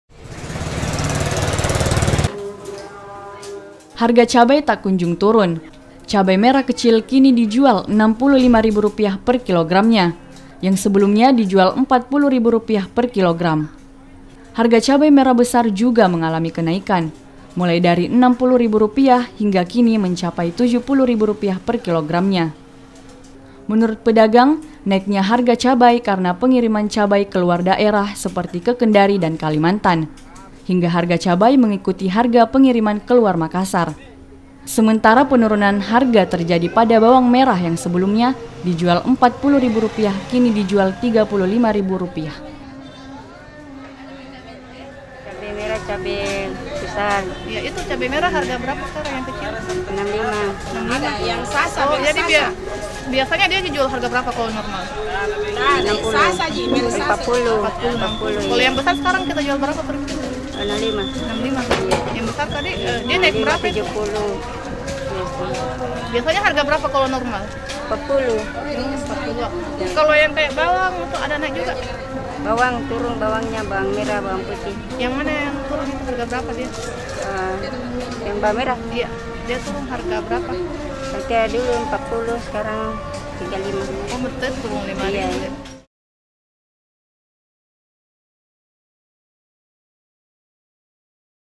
HARGA CABAI DI MAKASSAR MELONJAK NAIK
HARGA CABAI TAK KUNJUNG TURUN MESKI SEMPAT BERTAHAN DI HARGA 60 RIBU RUPIAH PERKILOGRAM. NAIKNYA HARGA CABAI LANTARAN BANYAKNYA PENGIRIMAN KE LUAR DAERAH.